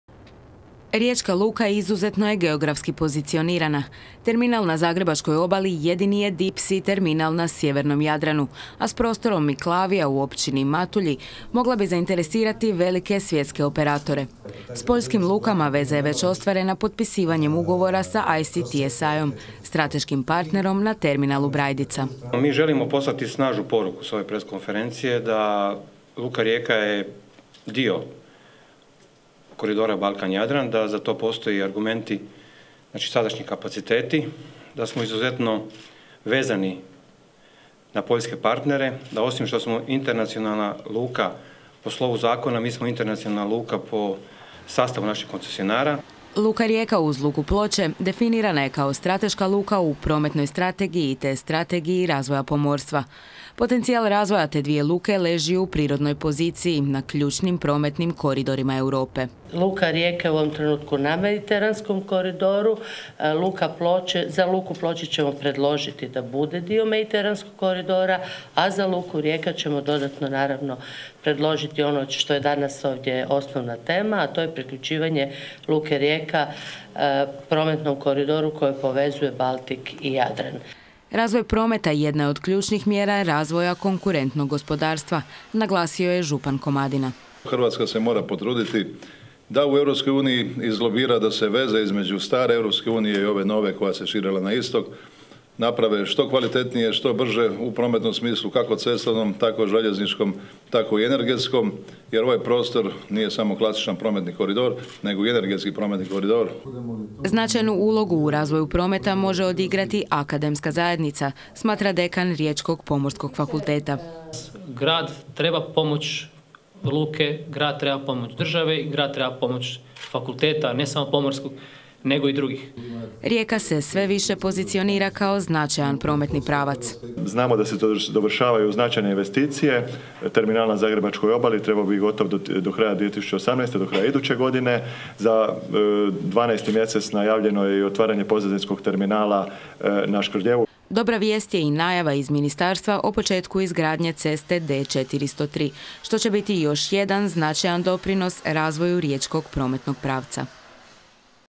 VELIKI INVESTICIJSKI CIKLUS
U sljedeće dvije i pol godine u razvoj riječke luke investirat će se čak 1,3 milijarde kuna